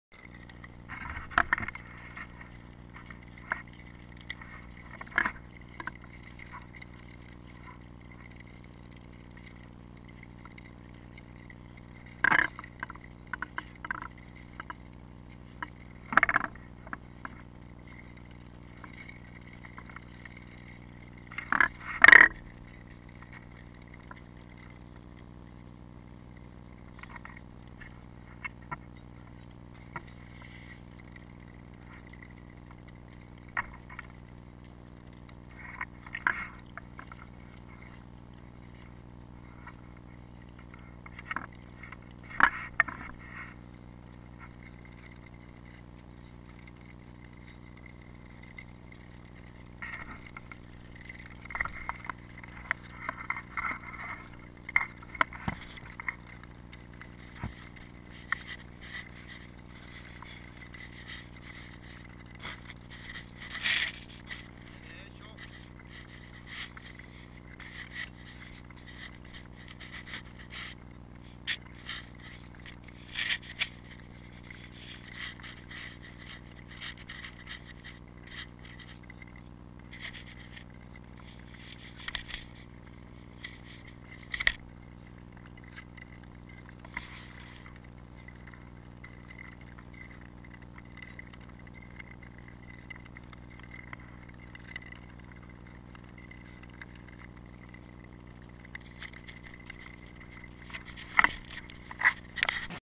Uppför Fyrisån
Det tog 18 år att komma till Ulva kvarn. Här passerar vi i kvällningen öppna stråk och Jumkilån.